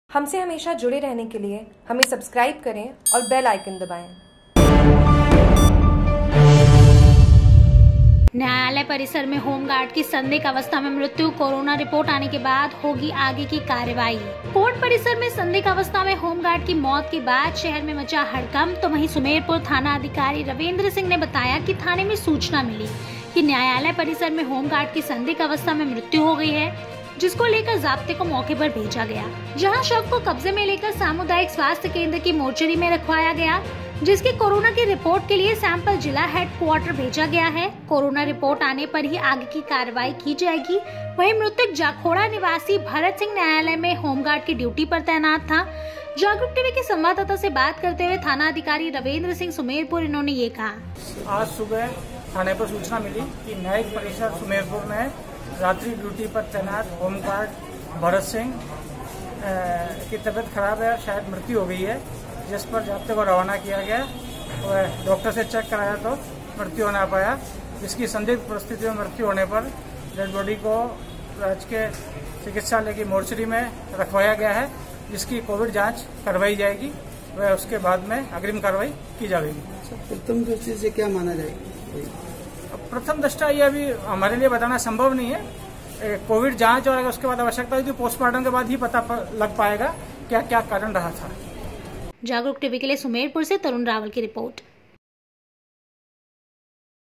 Jagruk TV सुमेरपुर : न्यायालय परिसर में होमगार्ड की संदिग्ध अवस्था में मृत्यु
सुमेरपुर- कोर्ट परिसर में सन्दिग्ध अवस्था में होमगार्ड की मौत के बाद शहर में मचा हड़कंप तो वही सुमेरपुर थाना अधिकारी रविंद्र सिंह ने बताया कि थाने में सूचना मिली कि न्यायालय परिसर में होमगार्ड की संदिग्ध अवस्था में मृत्यु हो गई है। जिसको लेकर जाप्ते को मौके पर भेजा गया। जहां शव को कब्जे में लेकर सामुदायिक स्वास्थ्य केंद्र की मोर्चरी में रखवाया गया। जिसकी    कोरोना की रिपोर्ट के लिए सैंपल जिला हेड क्वार्टर भेजा गया है। कोरोना रिपोर्ट आने पर ही आगे की कार्यवाही की जाएगी। वही मृतक जाखोडा निवासी भरतसिंह न्यायालय में होमगार्ड की डयुटी पर तैनात था।

बाइट थाना अधिकारी रविंद्र सिंह सुमेरपुर

सुमेरपुर से तरुण रावल की रिपोर्ट

Jagruk TV